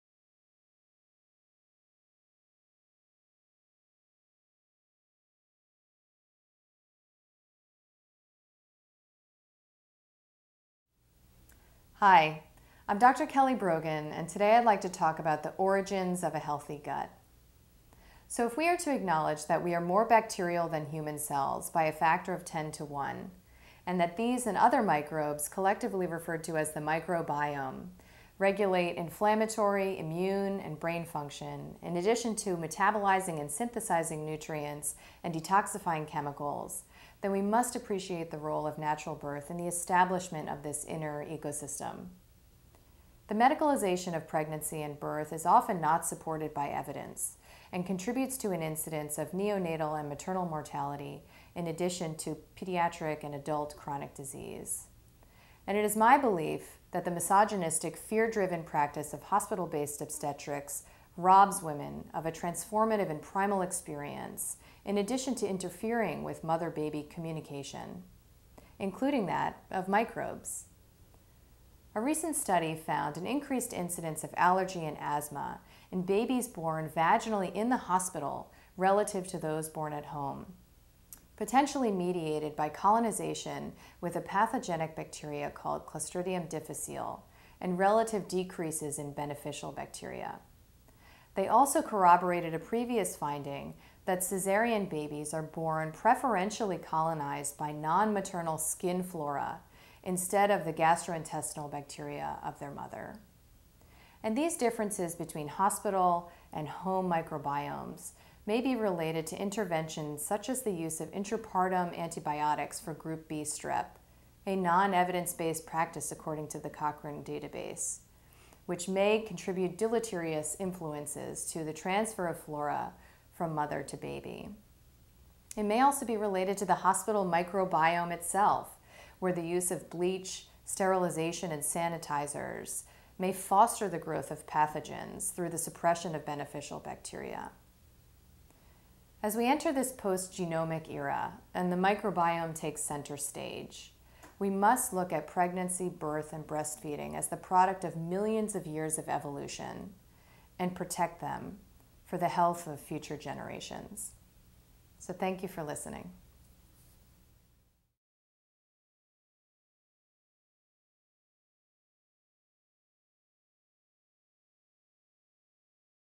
Origins of a Healthy Gut
Adult mental health has strong roots in the balance of flora in the body. How does a healthy gut get established? Learn about the role of birth in the microbiome.